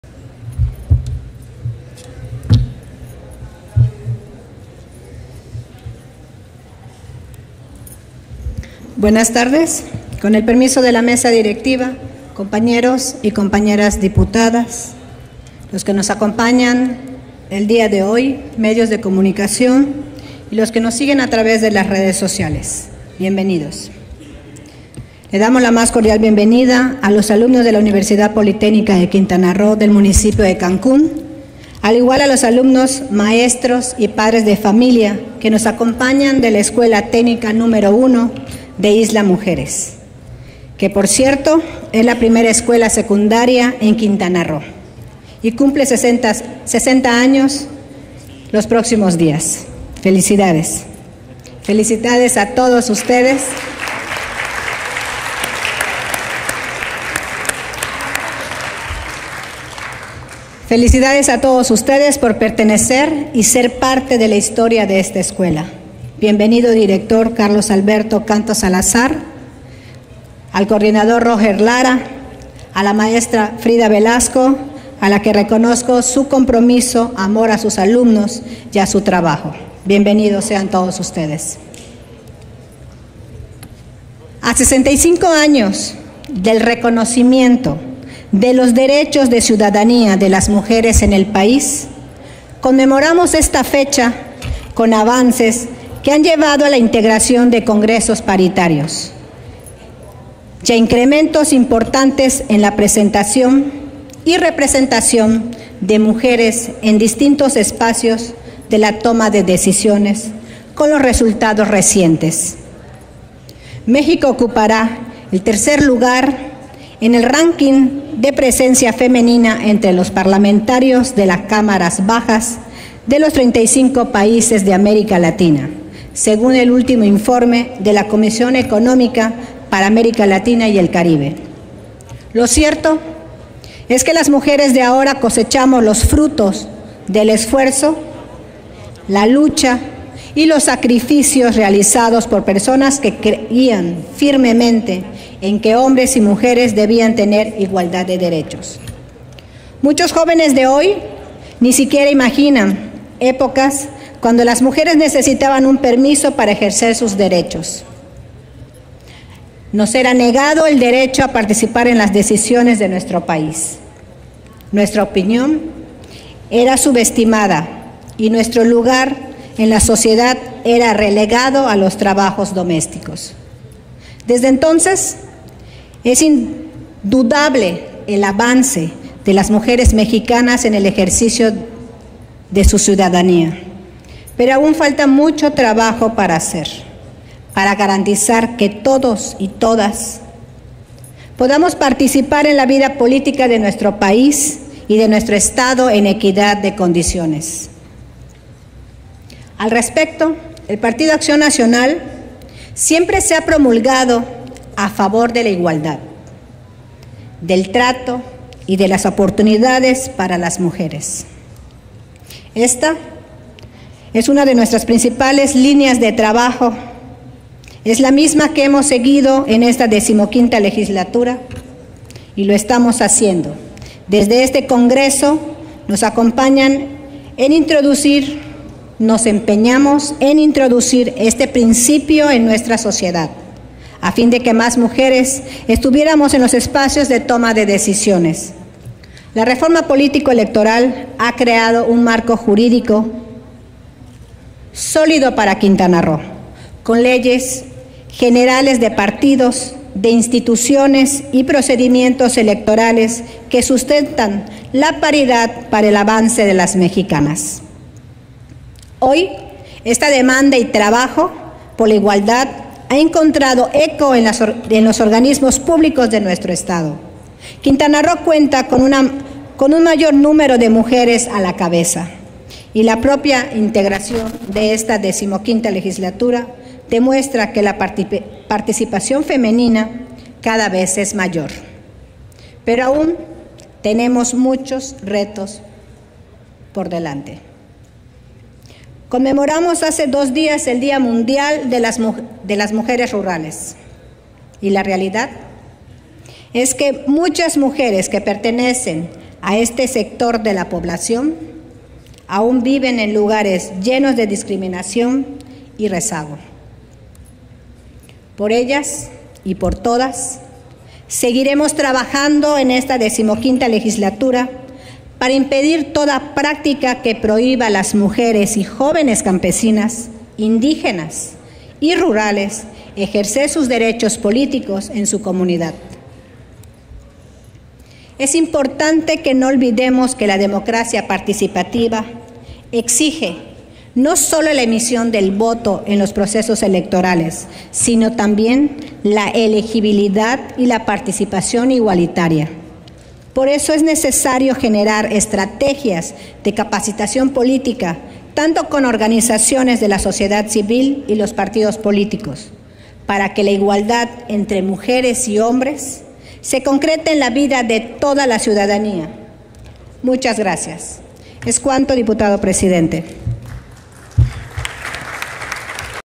Intervención de la Dip. Eugenia Solís Salazar. PAN (17/Oct/2018)
Intervención de la Dip. Eugenia Solís Salazar del PAN, en virtud de conmemorarse el “Día del Derecho al Voto de las Mujeres".